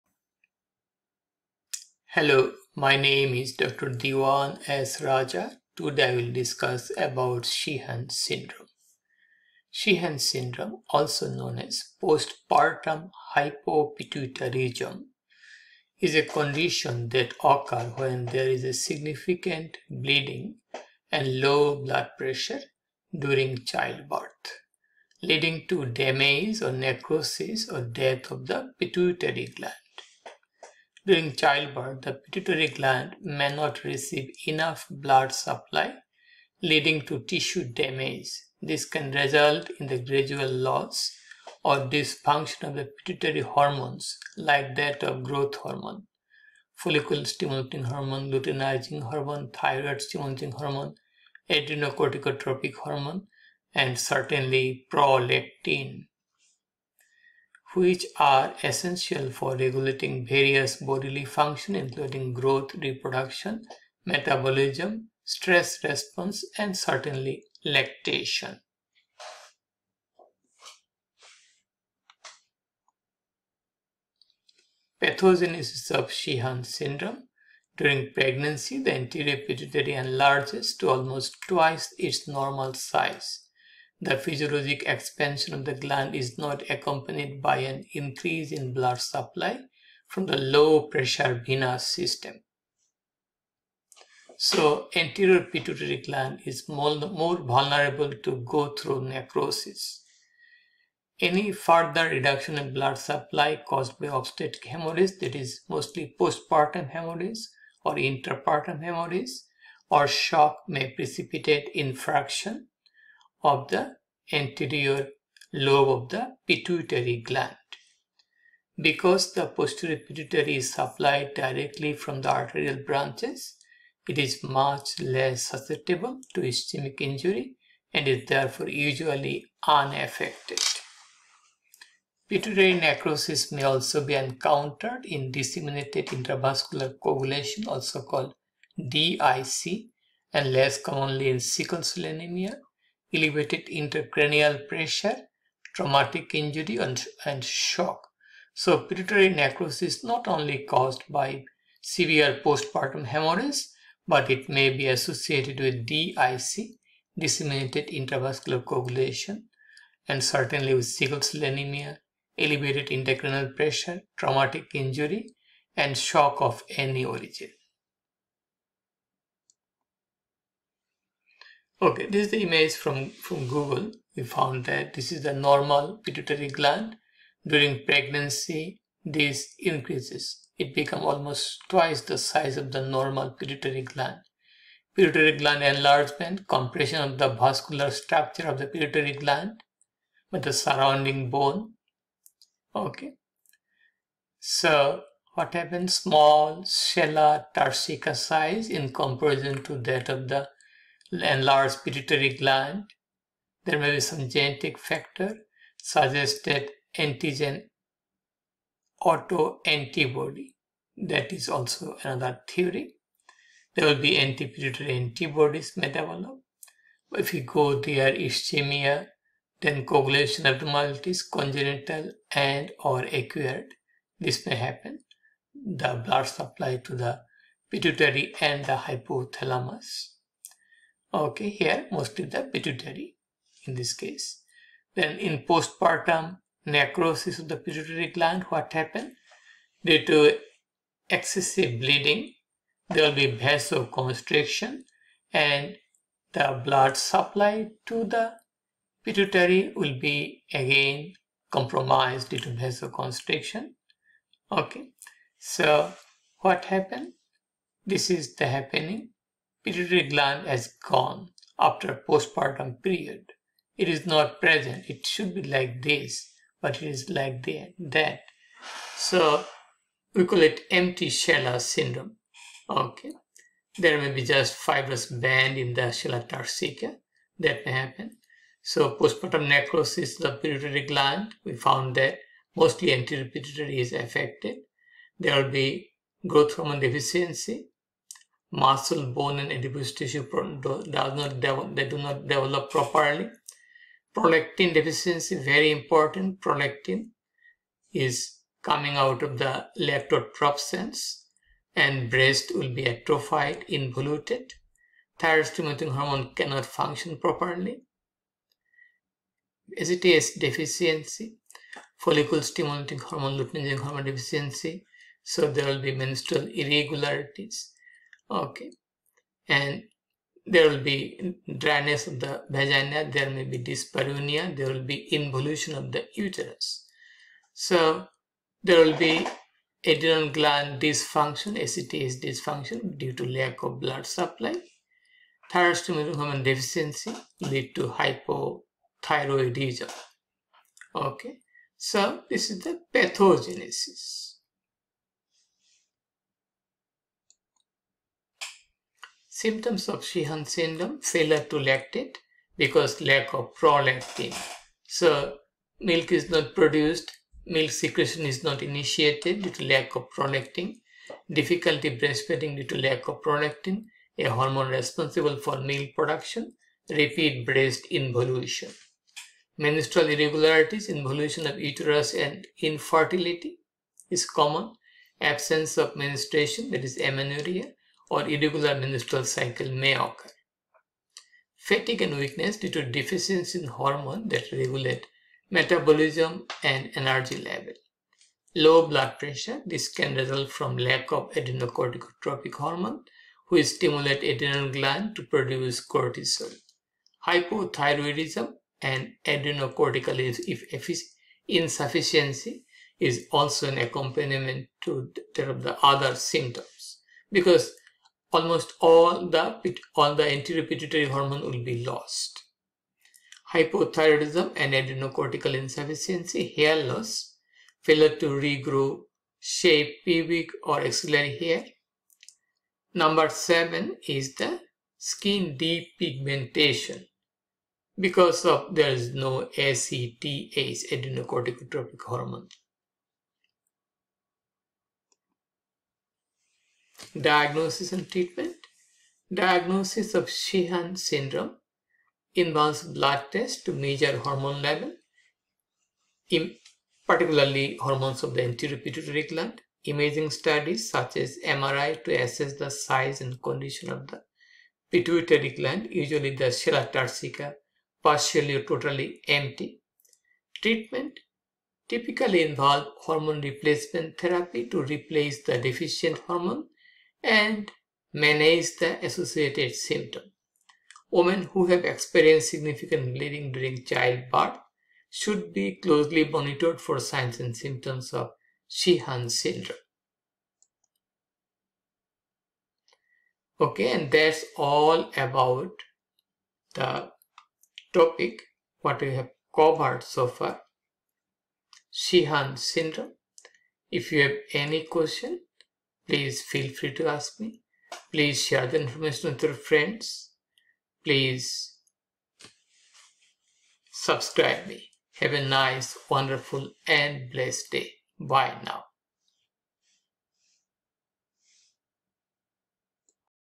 Sheehan's Syndrome
Definition
Etiology
Clinical Features
Anterior Pituitary Necrosis
Diagnosis
Treatment